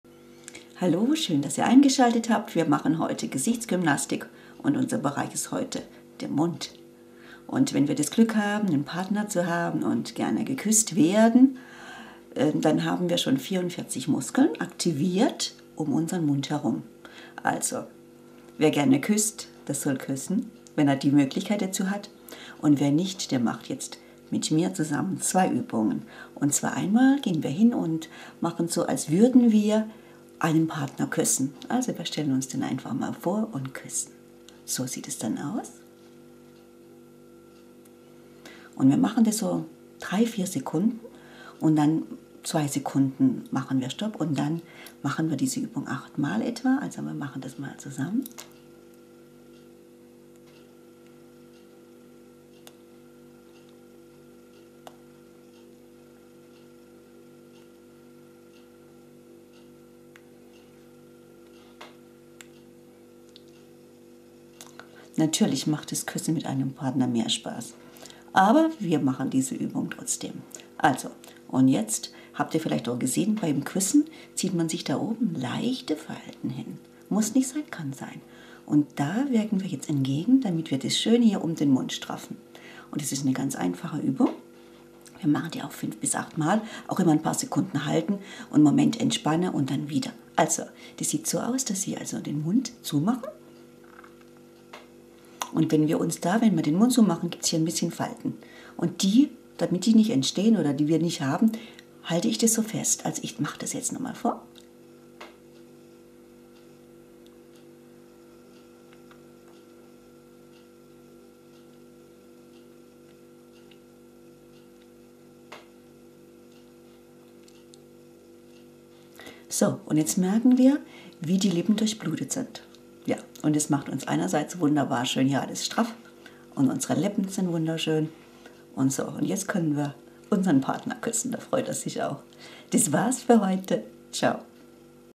Straffe Haut - Gymnastik - Mund - Bereich
Straffe Gesichtshaut in jedem Alter - mit einer effektiven regelmäßigen Gymnastik möglich - inspiriert durch das Buch Self Lifting von Camille Volaire